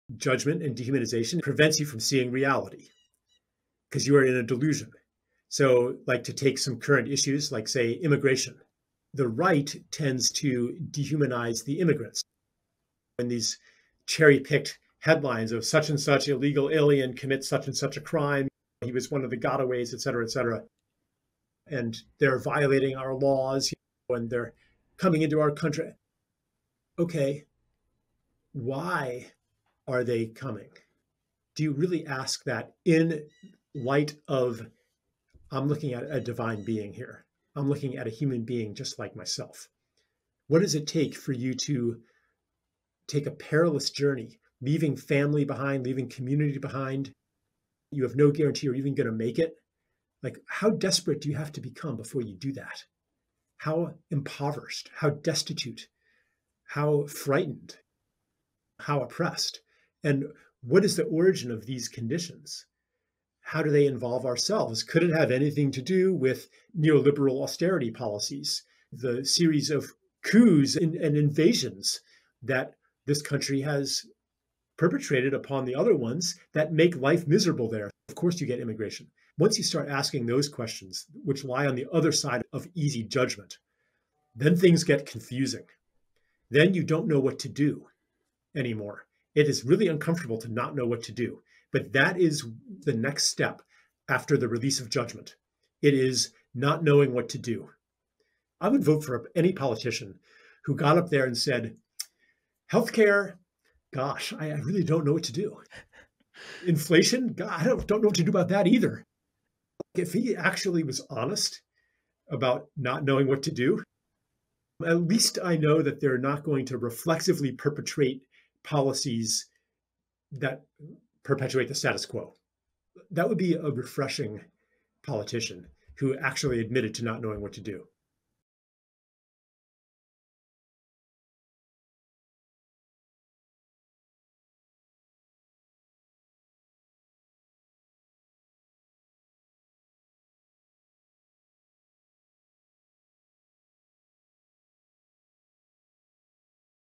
Charles Eisenstein on Seeing Complexity After Releasing Judgement
Join us in Austin, October 29-31 for the Independent National Convention.